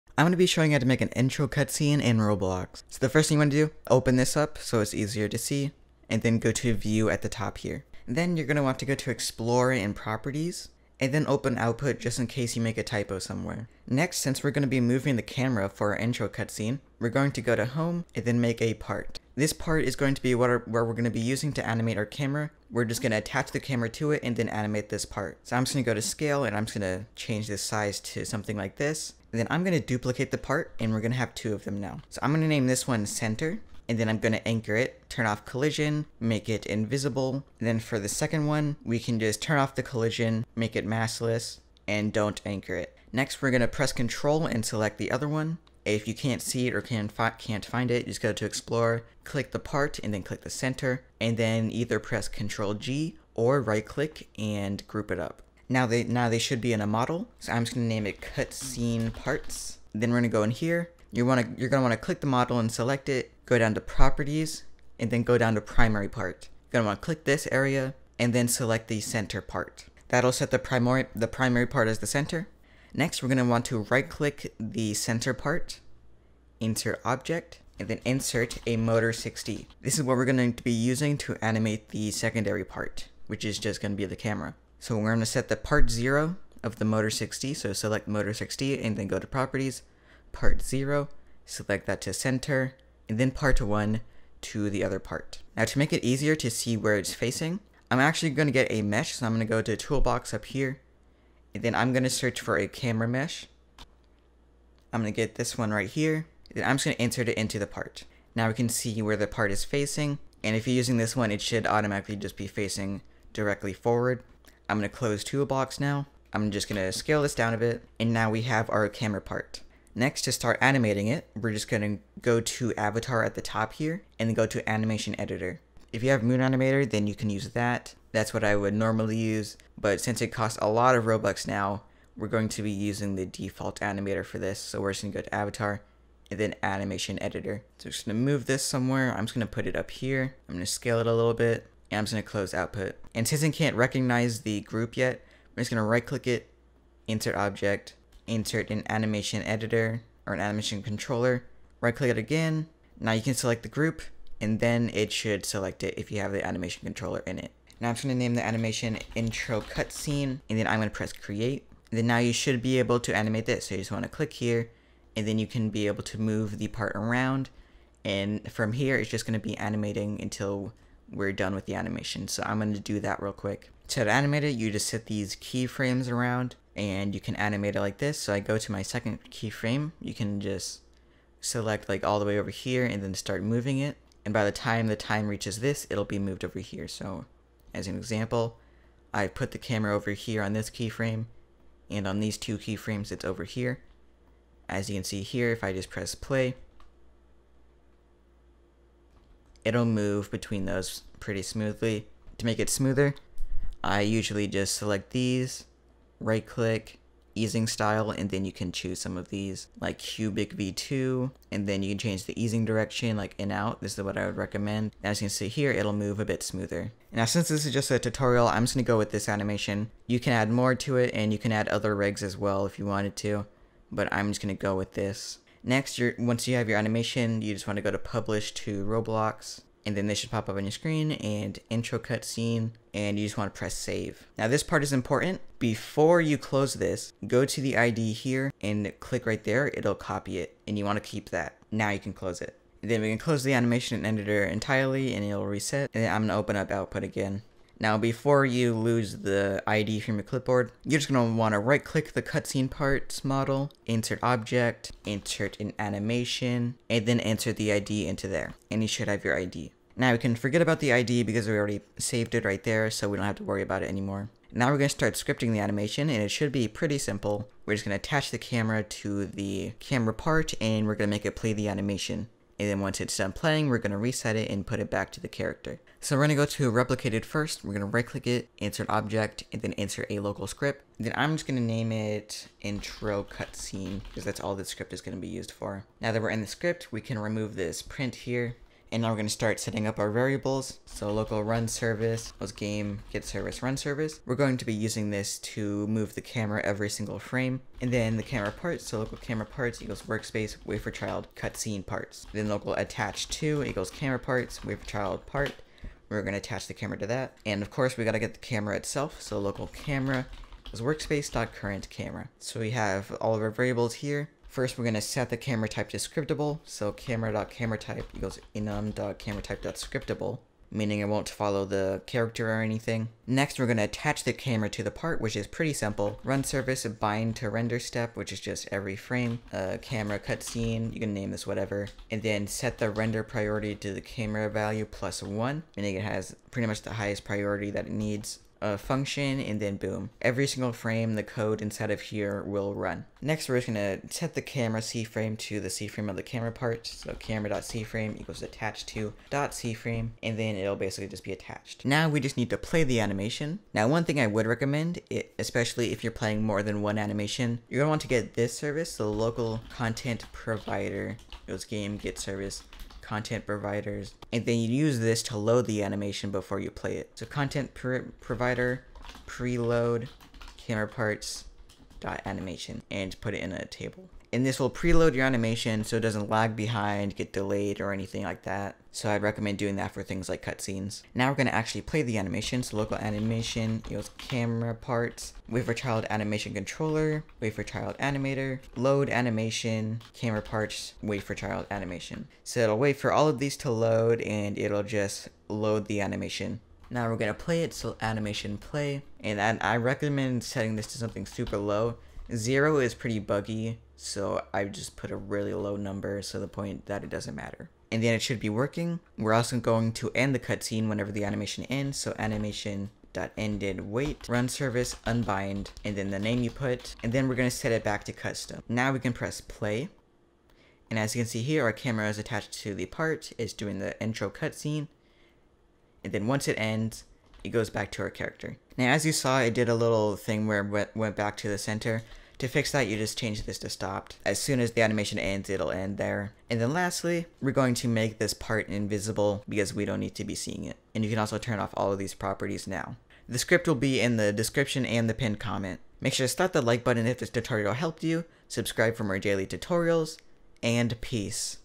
How to Make an Intro Cutscene (ROBLOX STUDIO)
In this video I show you how to make an intro cutscene in Roblox Studio that lets you animate the camera!

Script:
local runService = game:GetService('RunService')
local cameraParts = workspace:WaitForChild('CutsceneParts')
local attachTo = cameraParts:WaitForChild('Part')
local camera = workspace.CurrentCamera

local contentProvider = game:GetService('ContentProvider')

camera.CameraType = Enum.CameraType.Scriptable

runService:BindToRenderStep('CameraCutscene',Enum.RenderPriority.Camera.Value+1,function()
 camera.CFrame = attachTo.CFrame
end)

contentProvider:PreloadAsync({cameraParts.Animation})

local animation = cameraParts:WaitForChild('AnimationController'):WaitForChild('Animator'):LoadAnimation(cameraParts:WaitForChild('Animation'))
animation:Play(0.01)

animation.Stopped:Wait()

runService:UnbindFromRenderStep('CameraCutscene')
camera.CameraType = Enum.CameraType.Custom